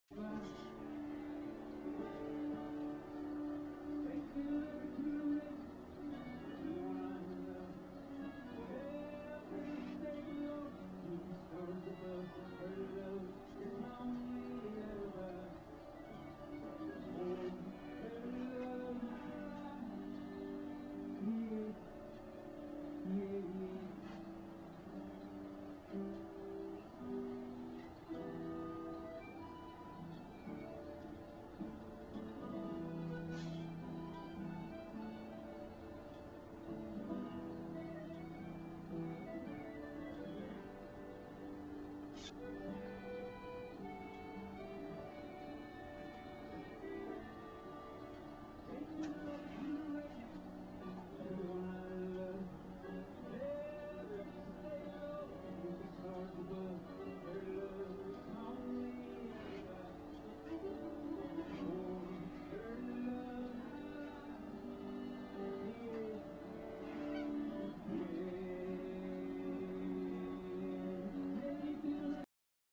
Zach Watches "Kitty TV"
Zach sat in the window for a long time watching the squirrels, blue jays and dark-eyed juncos, then looked at me as if to say,"Mom, can I go out and chase them?"